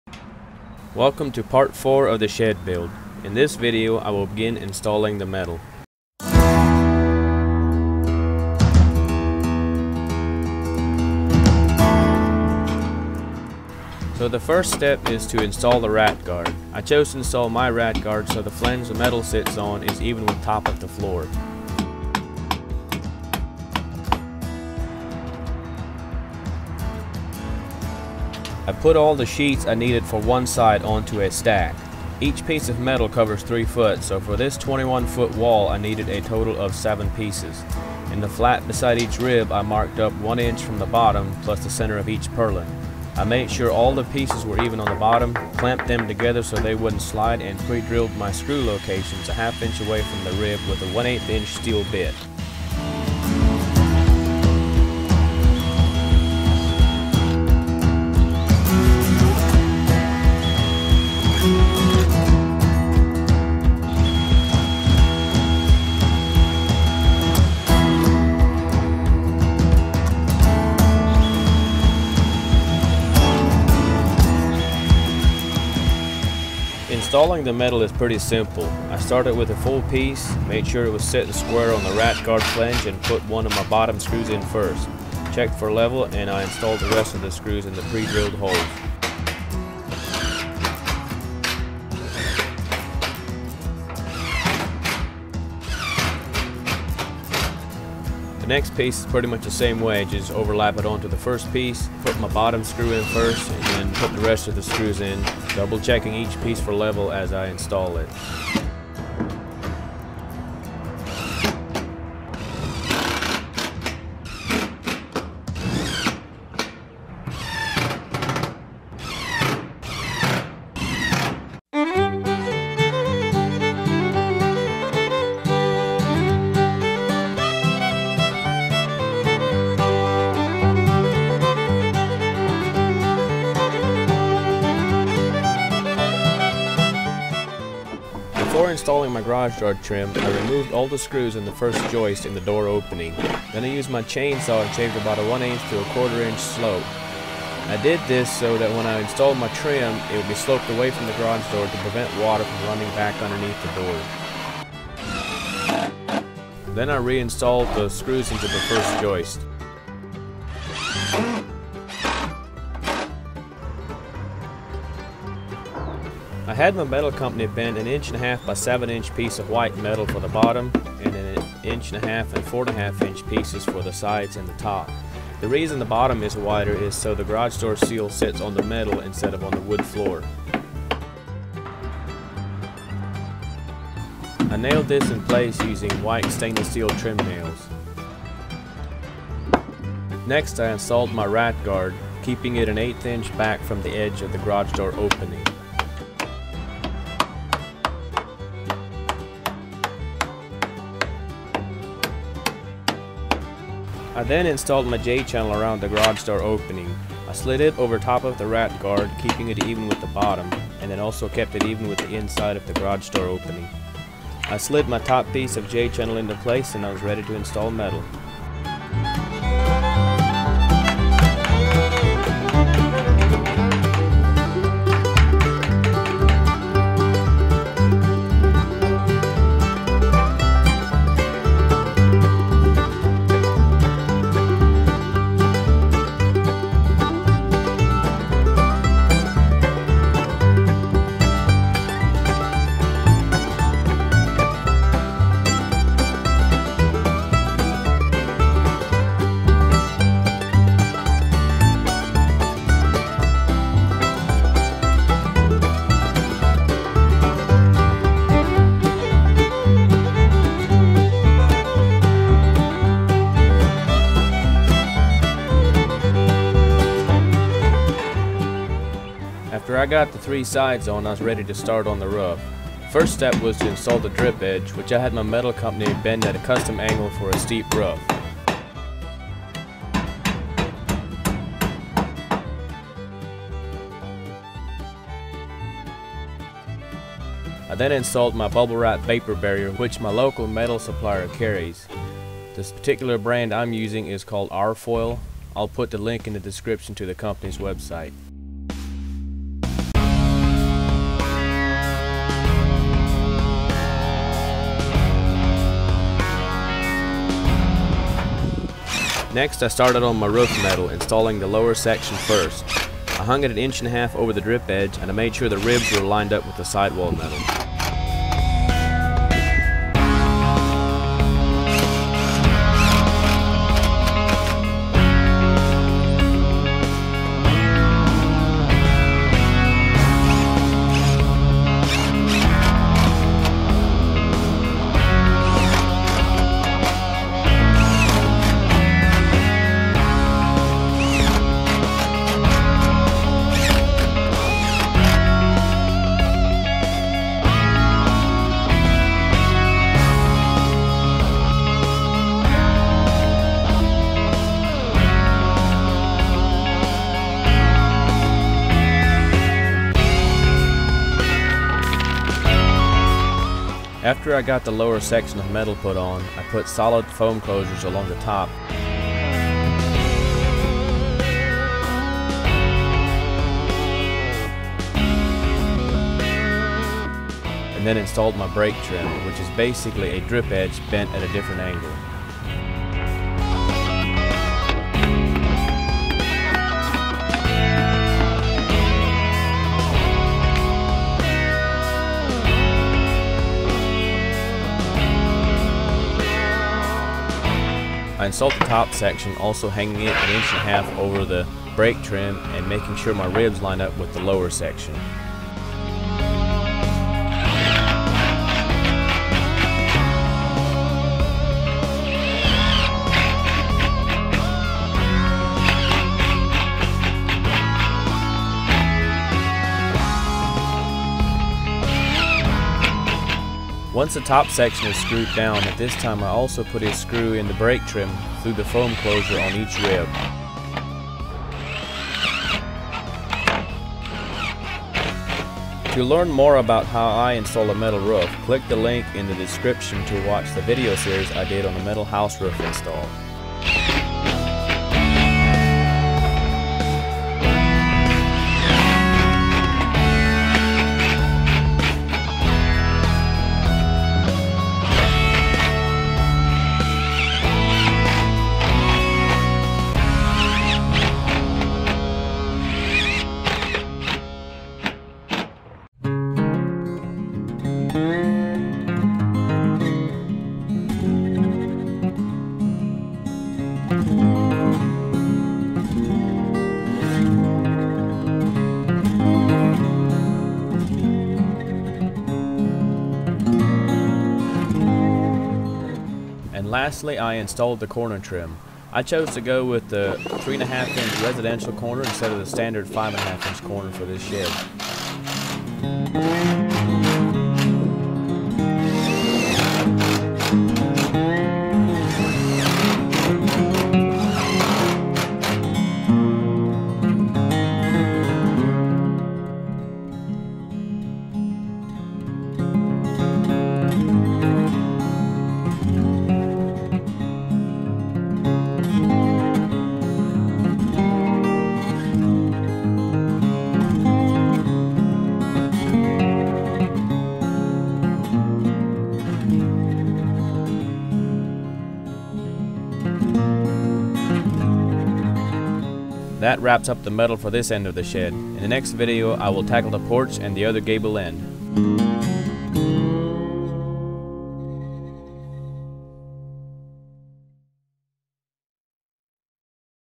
HOW TO BUILD A SHED
This is part 4 of a 5 part video series on building a shed. In this episode I install metal on 3 sides and the roof including the trim, corners, and ridge cap.

This shed was built in spring of 2019 and the total cost of materials was $3500

Here is the materials list.

Part 1
   10- 4X8X16 concrete blocks   (pads)
   12- 2x6x12' treated   (for runners/beams)
   24- 2x4x12' treated  (floor joist and girth boards)
   8 pcs 4'x8'x3/4" ADVANTECH subfloor (available at lowes)
   5 lbs 3" deck screws
   5 lbs 2" deck screws

Part 2
   14- 2x4x12 ’ regular (Top & bottom plates + perlins)
   16- 2X4X10’ regular (Top & bottom plates + perlins)
   42- 2x4x8’    regular (Studs + bracing)
   2- 4x4x8’   Cedar-tone treated (porch header / loft floor) (Available at Menards)
   3- 2x8x12’ Cedar-tone treated (porch header / loft floor) (Available at Menards)
   1-2x8x8’    Cedar-tone treated (porch header / loft floor) (Available at Menards)
   10 lbs 16 penny nails
   2 lbs 3” deck screws

Part 3
   26-2X4X8'                        (rafters)
   2 pcs 4x8x7/16" O.S.B. (rafter plates)
   6 - 2X4X12'                     (spacer perlins)
   11-2x4x16'                      (roof perlins)
   1 - 2x4x12'                      (roof brace)
   5 - 2x4x10'                    (end-wall framing)
   3 - 2x4x8'                       (end-wall framing
   1 - 2x4x12'                      (end-wall perlins)
   1 - 2x4x10'                      (end-wall perlins)
   4 - 2x4x8'                       (end-wall perlins)
   5 lbs 16 penny nails      (framing)
   3 lbs 8 penny nails        (rafter plates)

Part 4
Roof and trim = (CHARCOAL Color)(crinkle texture)
16 pcs metal 66”          (upper part of roof)
16 pcs metal 30”           (lower part of roof)
4- 8’x3 ½” corners       (Shed wall corners)
4-10’x3 ½” corners         (ends of the roof)
6 pcs rat guard  (bottom of sidewall metal)
8 pcs j-channel               (around the doors)
5 pcs drip edge 4”x4” (bent for a 24/12 pitch)     (roof eave)
5 pcs break trim 4”x4” (bent for a 10/12 pitch)   (break between upper and lower roof)
3 pcs ridge cap                  (peak of roof)
1 bag of 2” Charcoal screws (250)               (to fasten the roof metal)
1 bag of 1” Charcoal screws (250)               (to fasten corners)
32 solid foam closures     (under break trim and ridge cap)
1 – 4’x125’ roll of bubble-wrap                (roofing underlayment)

Sidewall metal = (IVORY color)
2 pcs metal 120”        (each side of garage door)
3 pcs metal 85”              (each side of entry door)
14 pcs metal 83”                (both sidewalls)
4 pcs metal 54”            (2 center pieces on each gable end)
2 pcs metal 3’       (2 outer pieces on gable above porch)
2 bags 1” Ivory screws (500)                  (sidewall metal)

Garage door trim = white
1 pc  1 ½”x7”x10’  (hemmed on both sides)    (bottom piece)
3 pcs 1 ½”x 4 ½”x10’          (sides and top pieces)

1 lb 1 ½” roofing nails                       (to nail trim on)
1 lb 1” white stainless steel trim nails     (to nail garage door trim and seals)

Part 5
Deck
9 - 5 1/2 X 10’ treated decking
2lbs 2 1/2” deck screws

Loft
9- 5 1/2” X 10’ treated decking
Or 1 1/2 pcs 4’ X 8’ Advantech
2lbs 2 1/2” deck screws

8’ wide X 7’ tall Garage Door
2-7’ garage door seals
1-8’ garage door seal
Garage door latch

1-36” Entry door (left hand)
Doorknob/deadbolt combo
Pack of shims

2- 24” wide X 36” tall windows
1-18” wide X 24” tall (loft) window
1 lb  1 1/2 roofing nails

Secondary loft  (light weight storage)
7-2x6x12’.                      (Loft Joists)
2-2x4x8’ (cut in 12” for blocks to support 2x6)
3 pcs 7/16” osb              (Loft floor)
18- 7/16” h clips
1 lb 2 1/2” screws
1 lb 1 1/4”!screws

Electrical
3 single nail on boxes
2 light boxes
1 plug-in & cover
2 3way switches & 2 single covers
2 white lamp holders & bulbs
60’ 14-3 wire
10’ 14-2
Wire staples

MATERIAL LIST IS NOT GUARANTEED